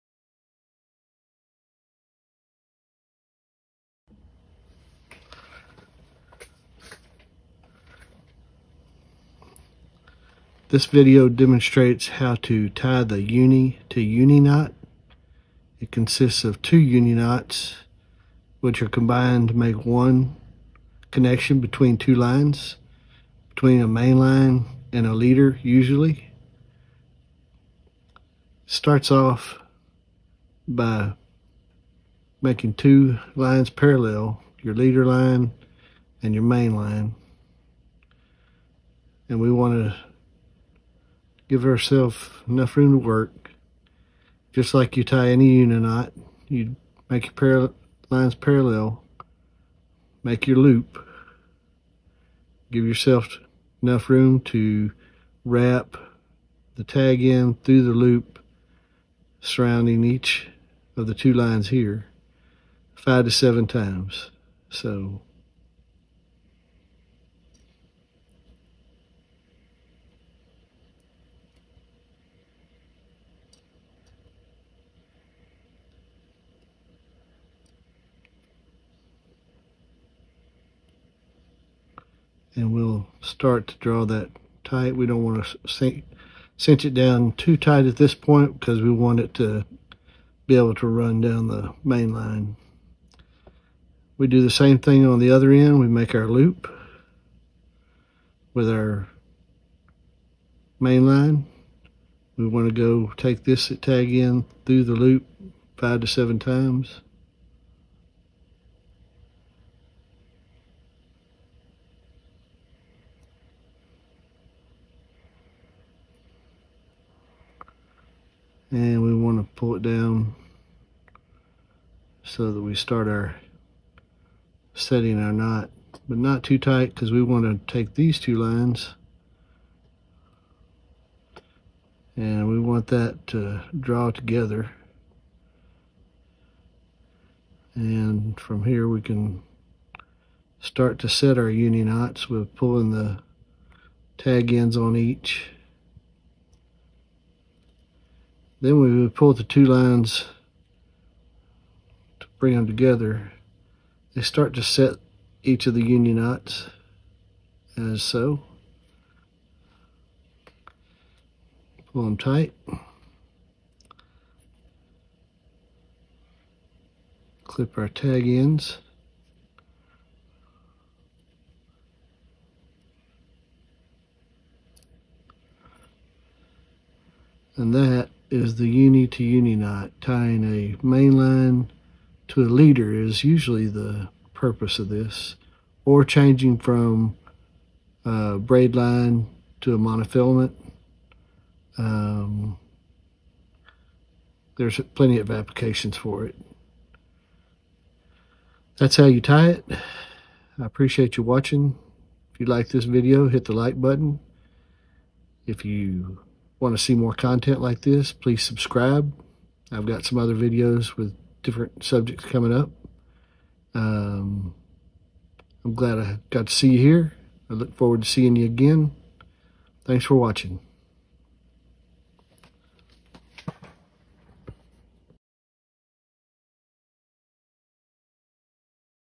The Double Uni Fishing Knot to Join Two Lines
The double uni knot or "uni to uni knot" is one of the best types of knots to use when connecting two lines of similar diameter.  Use this knot to join braid to mono or main line to leader.  This knot is one of the strongest line connecting knots you can use when learning how to fish.  Easy to tie.  These knots are required for rank advancement in Scouting BSA and should be a part of your skill set as well.  I hope you find this informative and useful knowledge in your outdoor experience.  More to come, soon!